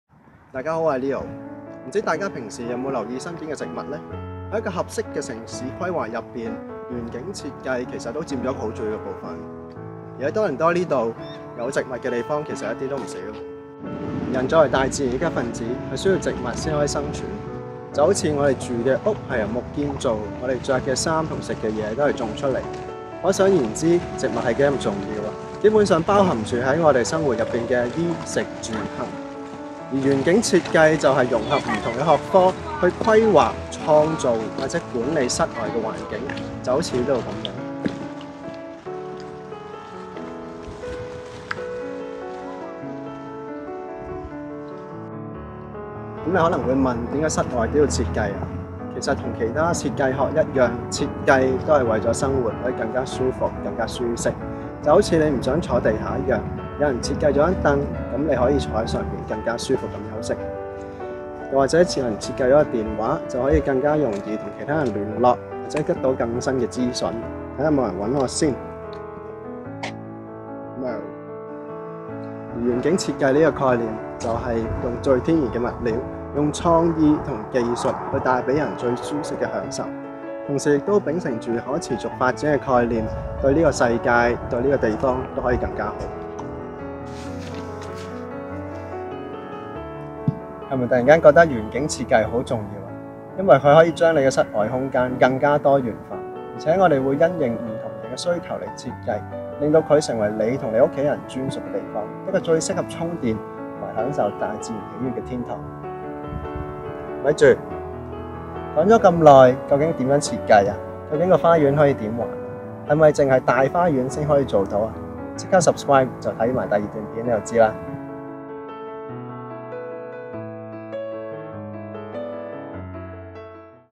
其實，甚麼是園境設計？｜What actually is Landscape Design?｜廣東話｜庭園意境
簡單來說，就是影片所說；
詳細來說，就是創造庭園意境。

為你設計，創造出理想庭園
成為你專屬的Green Magic Hours